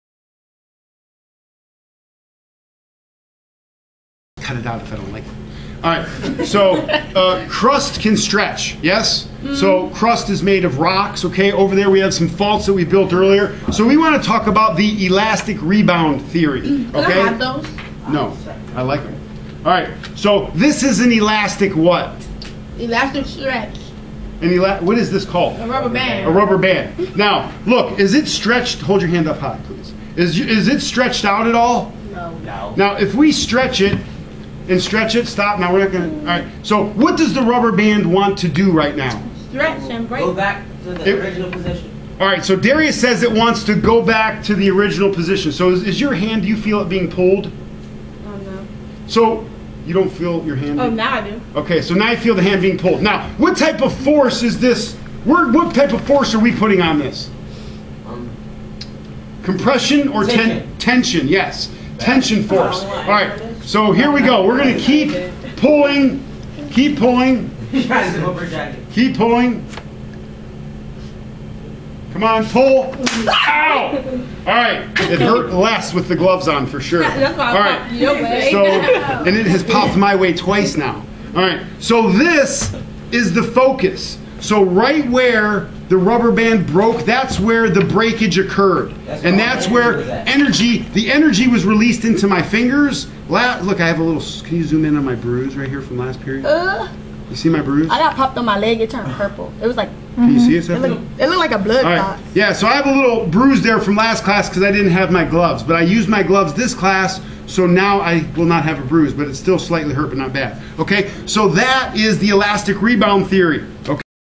Elastic Rebound Theory
This is one basic demonstration you can incorporate into a lesson to bring the elastic rebound theory to life.